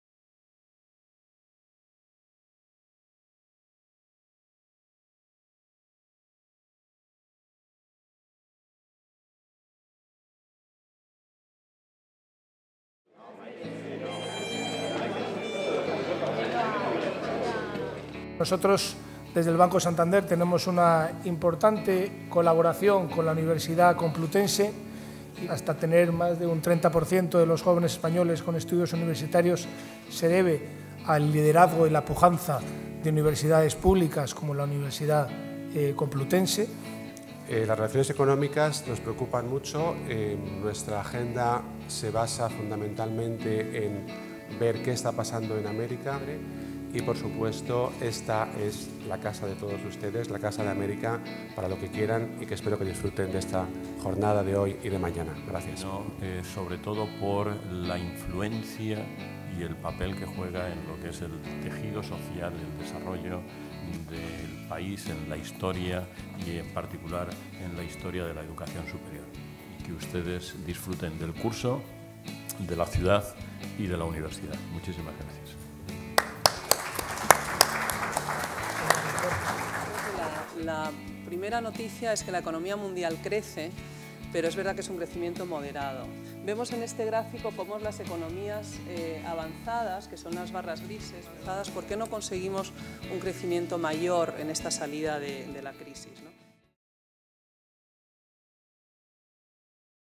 [CLIP] Cursos de Verano UCM 2016 - Casa de América
Clip resumen del acto de inauguración de los Cursos de Verano Complutense 2016 en su sede de la Casa de América. Grabación, edición y posproducción: Departamento de Estudios e Imagen Corporativa. UCM.